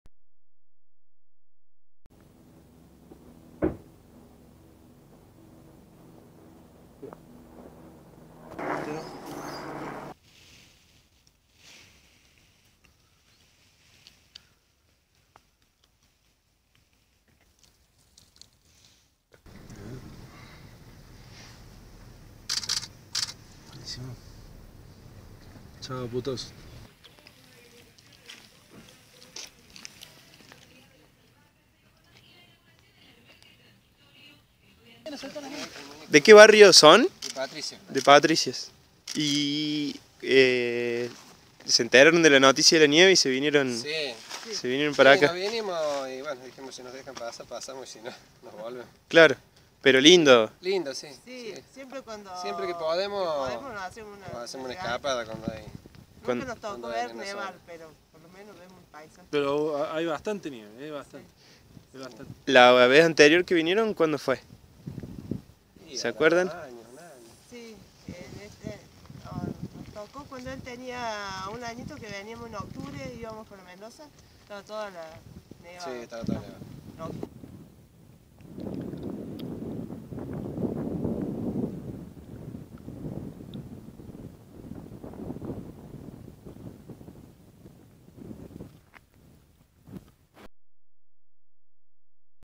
Nieve en Córdoba- El Camino de las Altas Cumbres
Nieve en Córdoba- El Camino de las Altas Cumbres. Mirá la nota con fotos y datos sobre la nevada en Córdoba